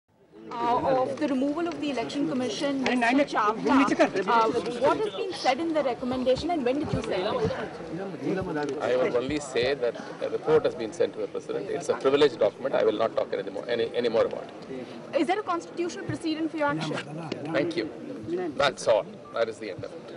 CEC Gopalaswami refuses to comment
Mumbai: Chief Election Commissioner N Gopalaswami whose recommendation to the government to remove Navin Chawla has created a storm today refused to comment on his report saying it is a privileged document.